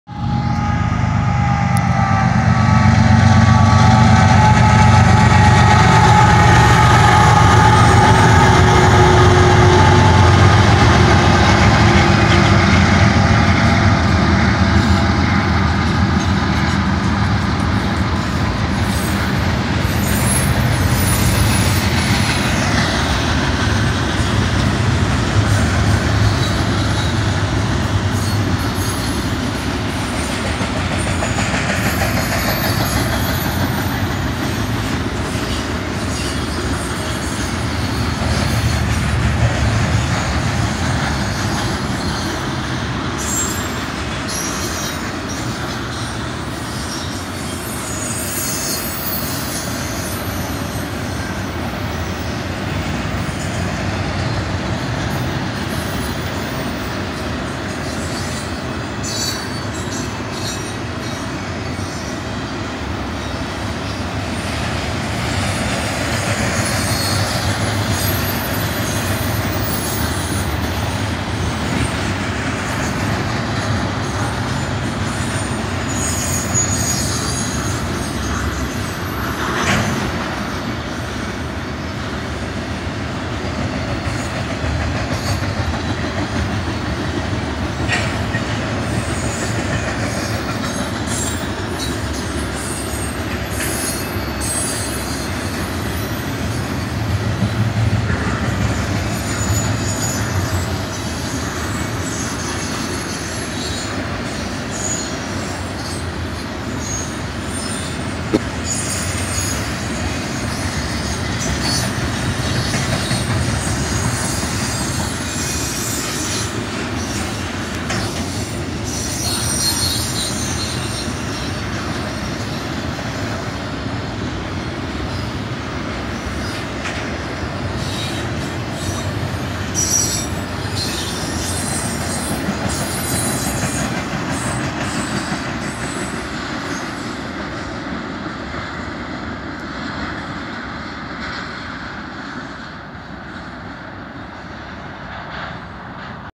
EB BNSF Domestic Intermodal Train At Lenwood Ca!
A eastbound BNSF domestic intermodal train went by here at Lenwood Ca!

Cajon Sub Milepost 4.9 Mainline 2